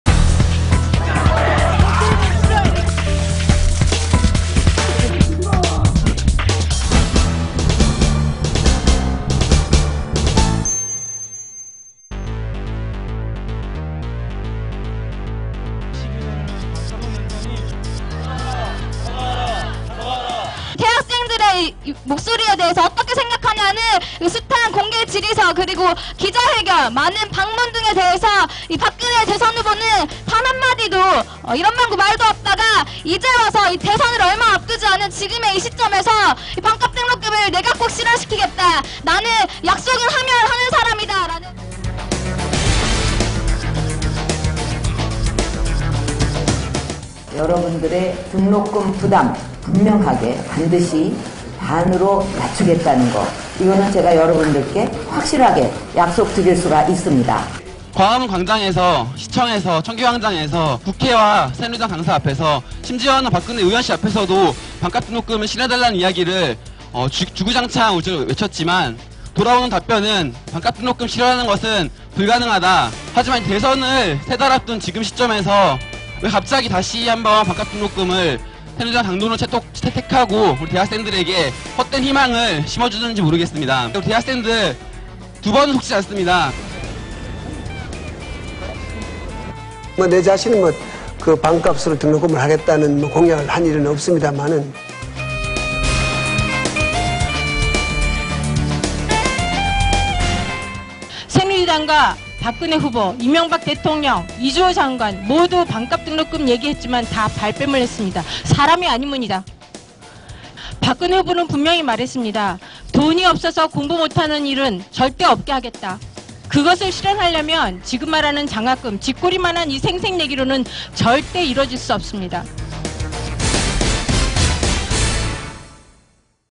[정PD가 보고 있다] "반값등록금 두 번 속지 않는다"
"여러분의 등록금 부담 분명하게 반드시 반으로 낮추겠다는 거. 이거는 제가 여러분께 확실하게 약속드릴 수가 있습니다." 지난 23일, 대학생 반값등록금 토론회에 참석한 박근혜 새누리당 대선 후보가 한 말이다. 언론은 박근혜 후보가 대학생들의 반값등록금을 공약으로 내세웠다고 일제히 보도했다.
이에 대학생들은 반발하고 나섰다. 지난 2007년 대선 때 새누리당이 내세운 반값등록금 공약을 너무도 싶게 이명박 대통령이 발뺌했기 때문이다. "나 자신은 반값으로 등록금을 하겠다는 공약을 한 일은 없습니다." 이런 경험 때문일까? 21세기 한국대학생연합(한대련)을 비롯한 반값등록금국민본부는 "두 번은 속지 않겠다며", "박근혜 후보가 반값등록금을 실현할 의지가 있다면, 지금 당장 9월 국회에서 관련 법안을 통과시켜야 할 것이다." 고 촉구했다.